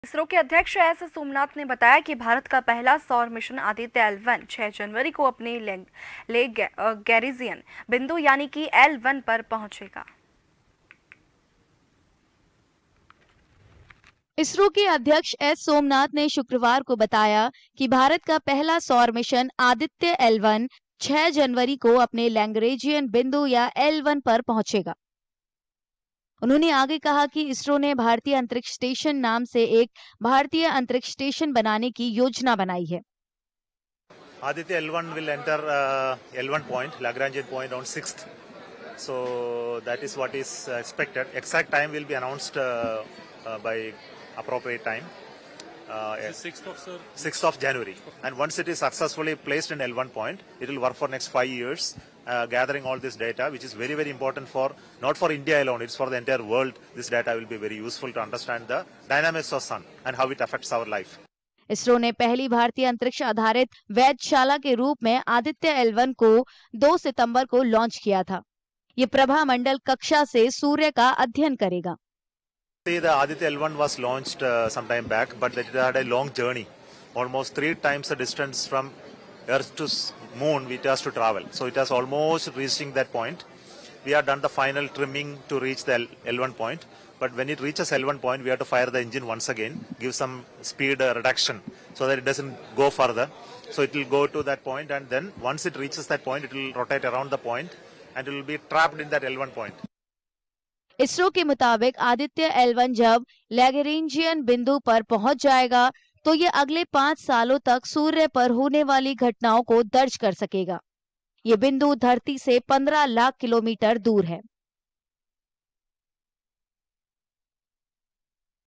Ahmedabad- भारत का पहला सौर मिशन है आदित्य एल-वन, आदित्य एल-वन 6 जनवरी को एल-वन प्वाइंट पर पहुंचेगा
Watch JAN TV on :
Tata Play DTH : 1185
Airtel DTH: 355
JIO Fiber: 1384

News Credit-SUS